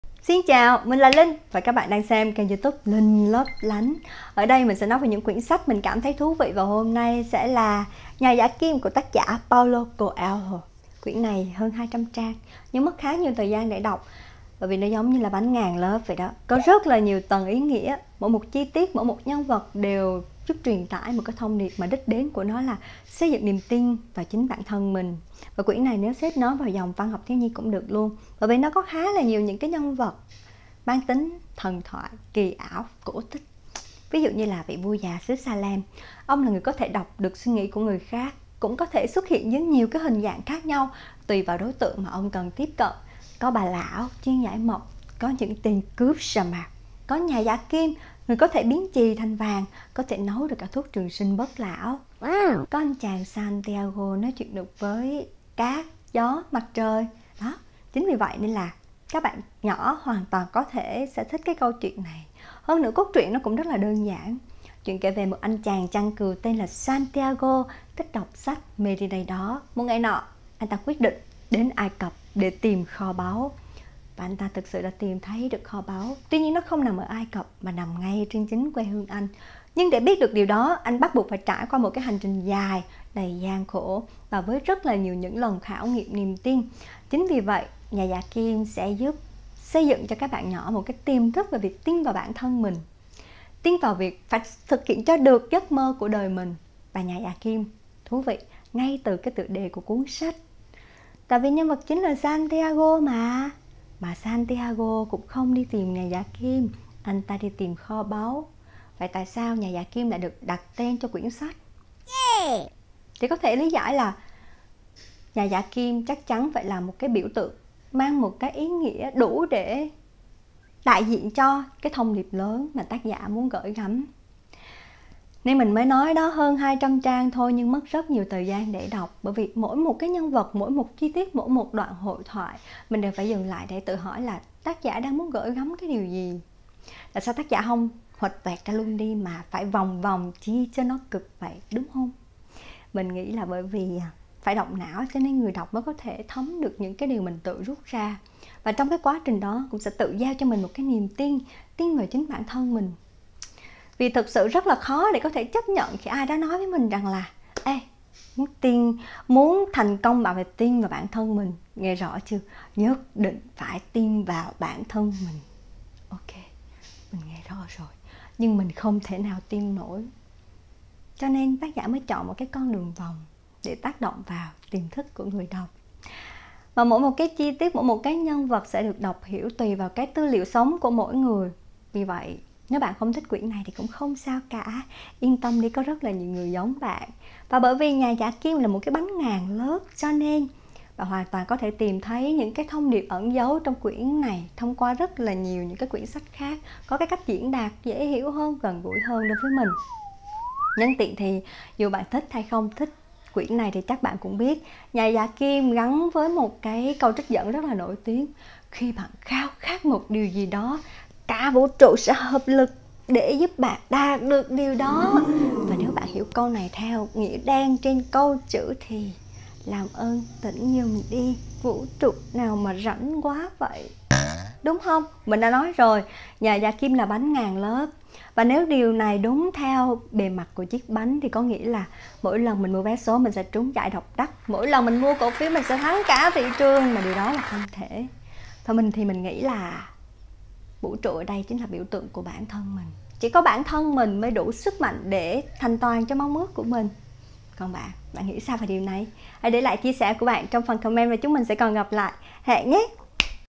⭐️[Review sách] Nhà giả kim 🐫 || Tác giả: Paulo Coelho|| Bánh ngàn lớp 🥐
🐫 “Nhà giả kim” của tác giả Paulo Coelho là quyển sách tạo cảm hứng cho rất nhiều người về việc theo đuổi ước mơ.
🕯  Chuyện kể về anh chàng chăn cừu Santiago, mộng thấy kho báu ở Ai Cập, cậu bán đàn cừu, để lên đường  👣tìm kho báu 💎. Con đường đi tìm kho báu đã giúp cậu học được nhiều bài học mới, cũng nhiều lần từ bỏ hy vọng rồi lại tiếp tục hy vọng về sự tồn tại của kho báu.

🧭  Những chỉ dẫn về cách đọc hiểu quyển sách đã được đưa ra ngay từ đầu trong câu chuyện cổ tích về chàng trai mang vẻ đẹp của hoa thủy tiên. Đáp án của nơi để kho báu cũng được ngụ ý ngay từ đầu. Tác giả đã rất dụng tâm trong việc giăng mắc rất nhiều tầng ý nghĩa để dẫn dắt người đọc tiếp cận với thông điệp. Nhưng cũng bởi vì có quá nhiều tầng ý nghĩa, “Nhà giả kim” trở thành tác phẩm không dễ để đọc hiểu. Tác giả đã chọn con đường vòng để dẫn dắt người đọc tin vào thông điệp, để cho hạt mầm niềm tin nứt vỏ trong chính trái tim mình. Và đó là cái giá mà ông phải trả cho dụng tâm của mình.

🍷  Chúc các bạn đọc sách vui vẻ và luôn tin vào trái tim mình.
💌 From Linhlaplanh with ❤️